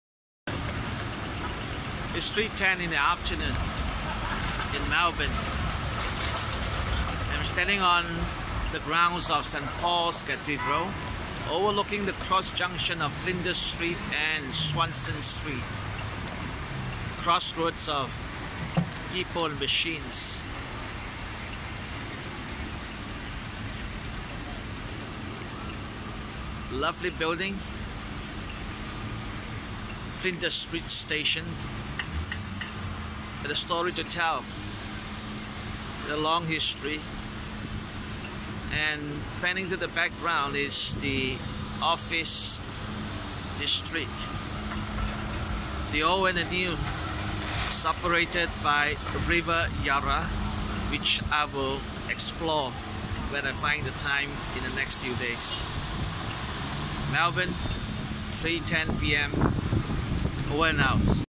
Flinders Street Station, Melbourne, Australia
Two famous Melburnian sayings, "I will meet you under the clocks" and "I will meet you on the steps" refer to the two famous features at the main entrance of the Flinders Street Station